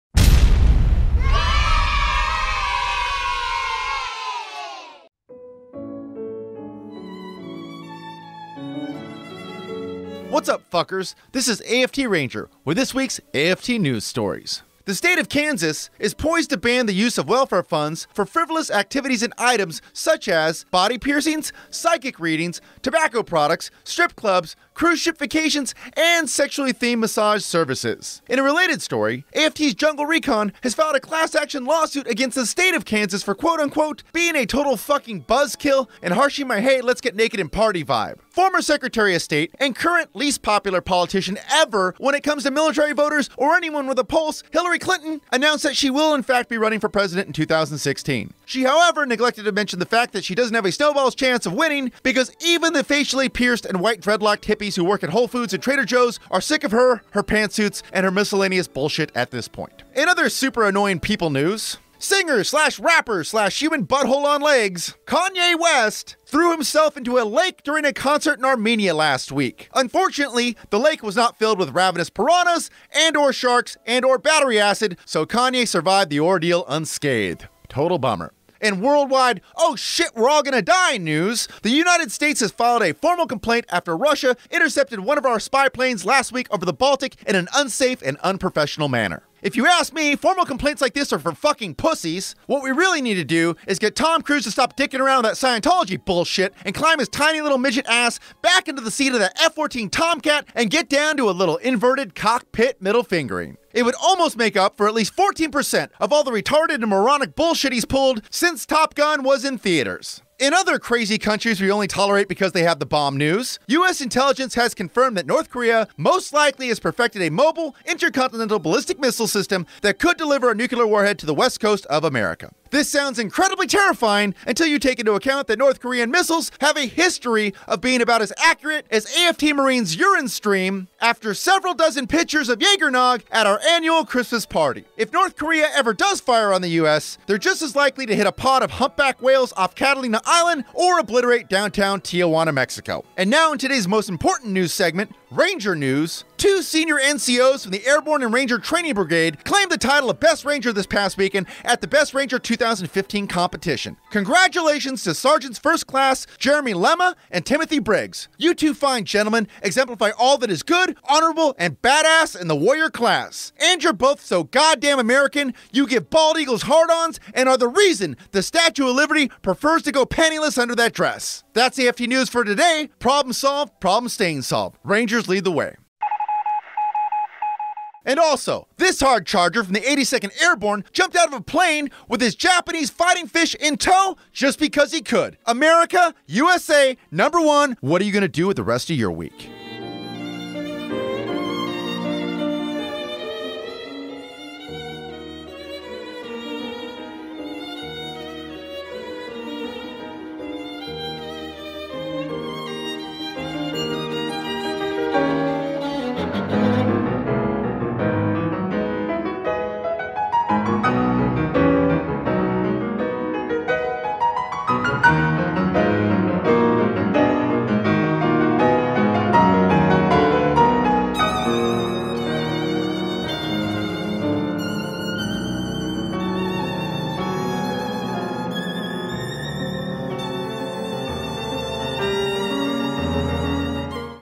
Kansas Outlaws Welfare Tattoos & Handjobs
Action Figure Therapy’s Angry Ranger Talks About Kansas Planning To Outlaw Welfare Tattoos & Handjobs.

It's AFT News. Your Source For Commentary On Dumb News Stories.